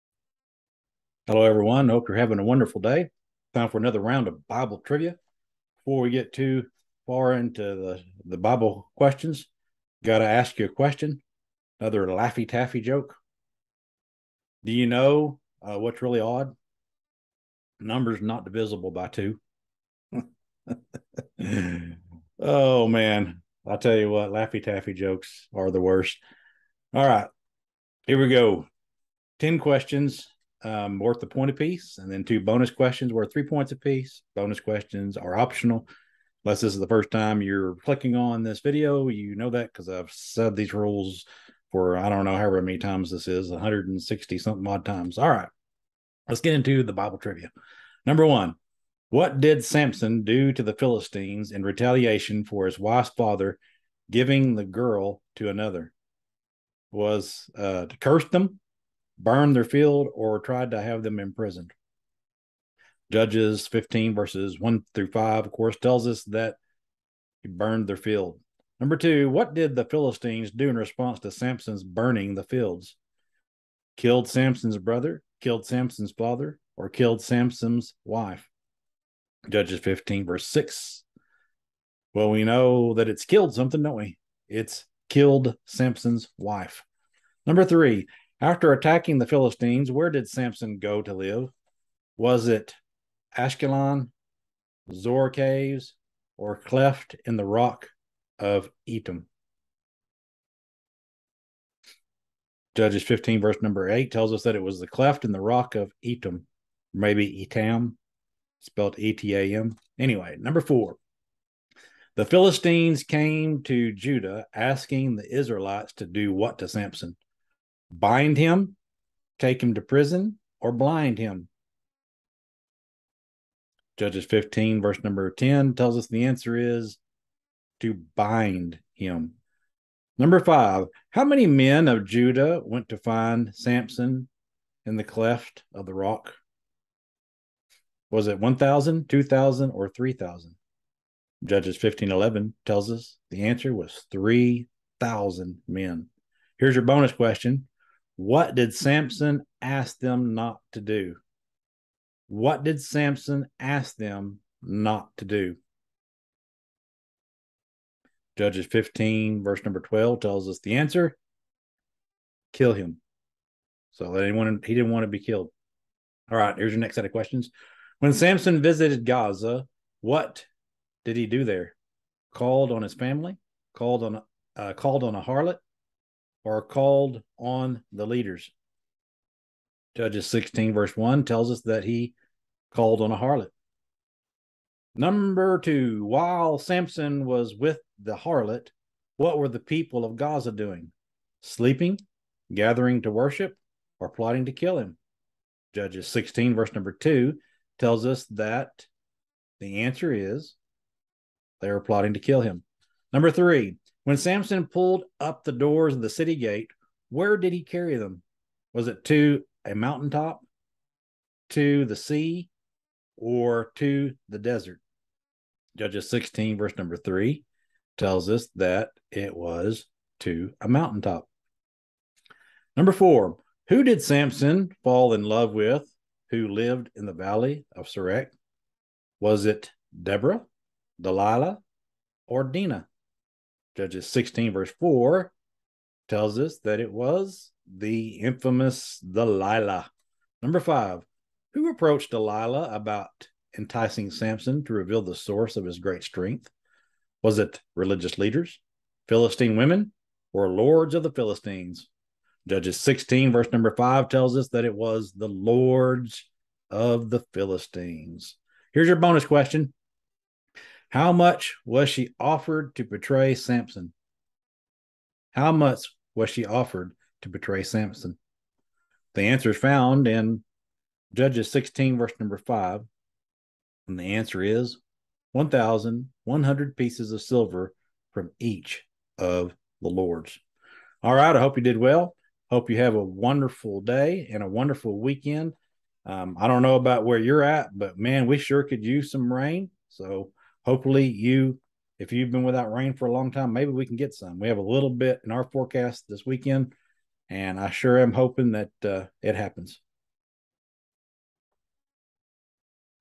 Bible Triva 162
I hope these short videos will help pass some time. You can challenge yourself or compete against your family members. There are 12 questions with two of them being bonus questions. Tell you how you did in the comments.